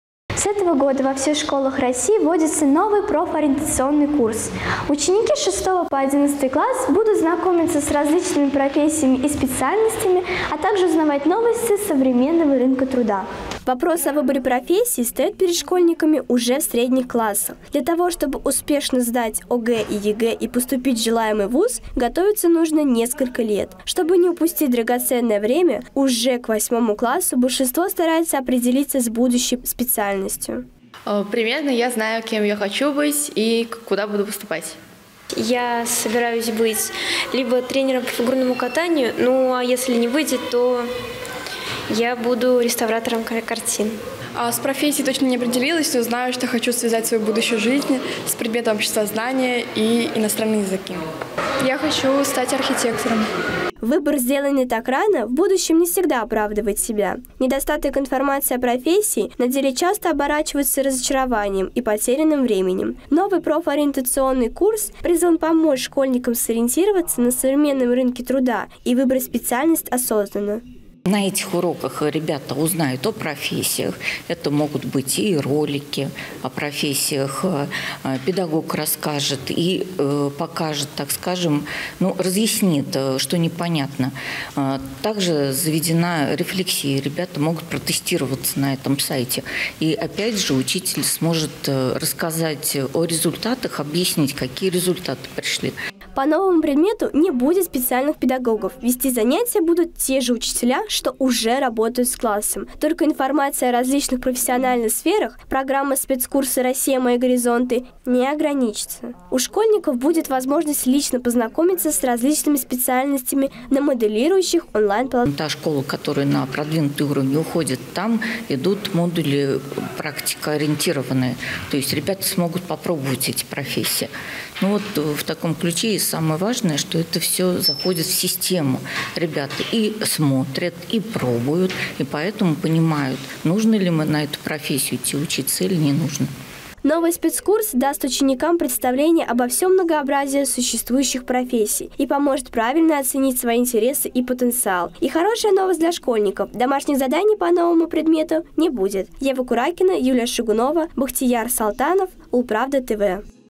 Мои горизонты. Ульяновским школьникам помогут определиться с профессией
С сентября во всех российских школах внедряется единая модель профориентации «Россия — мои горизонты». В Ульяновской области занятия по спецкурсу будут проходить каждую неделю по четвергам. На первом уроке по новой дисциплине побывали юнкоры детской редакции УлПравда ТВ.